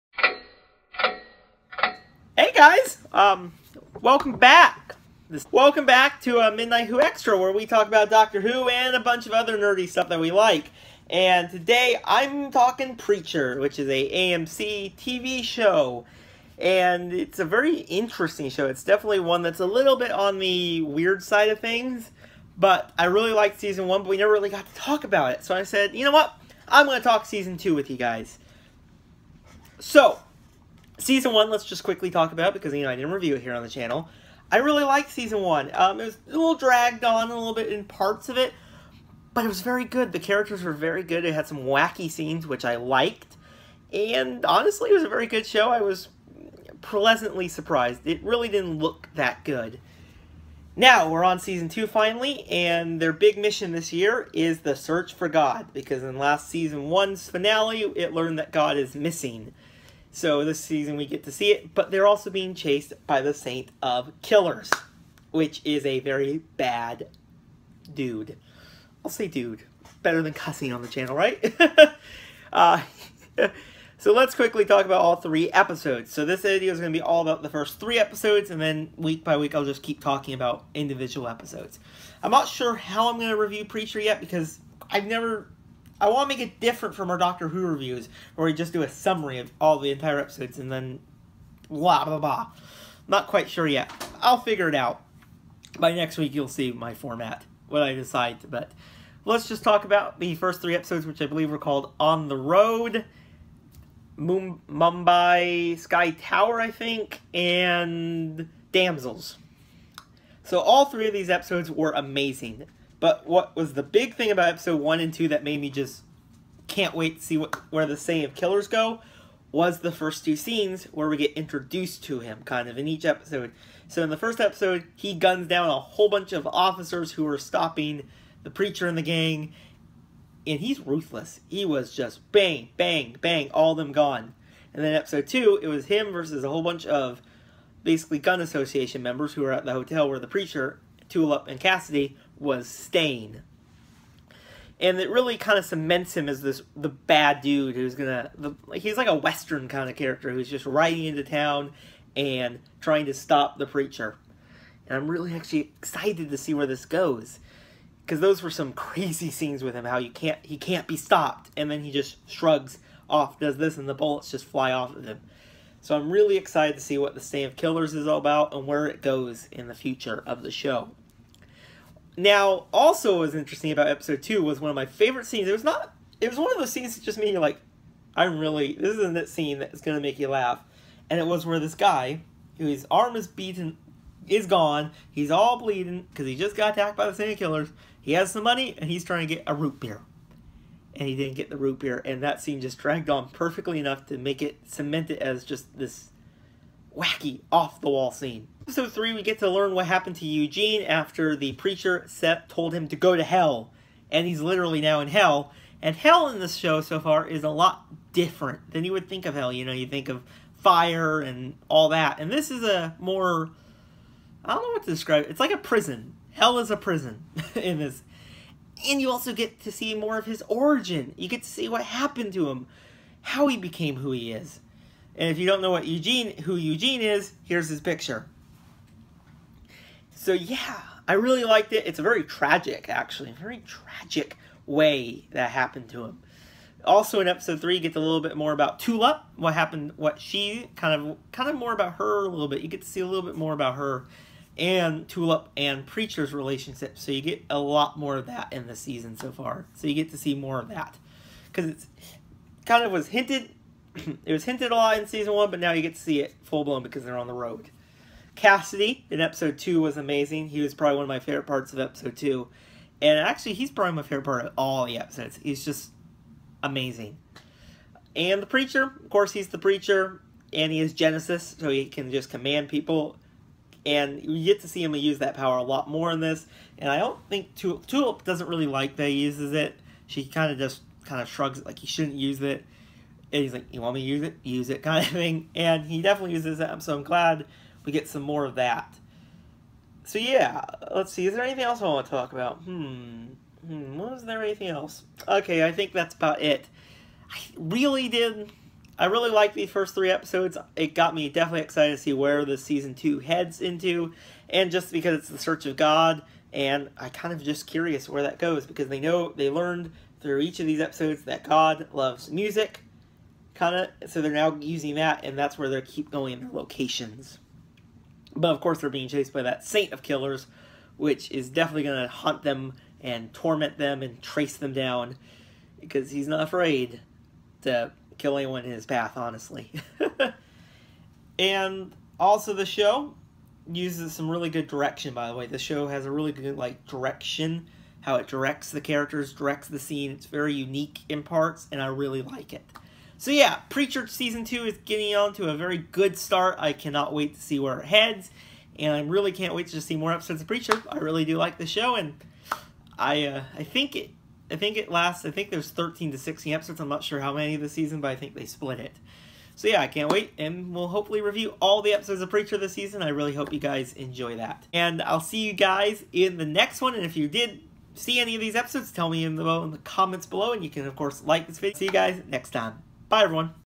Preacher REVIEW - Season 2 Episode 1-3
The Preacher Gang are on the road to find God. On the way they get chased by The Saint of Killers.

In this episode of Midnight Who Extra, the Professor reviews the first three episodes of Season 2.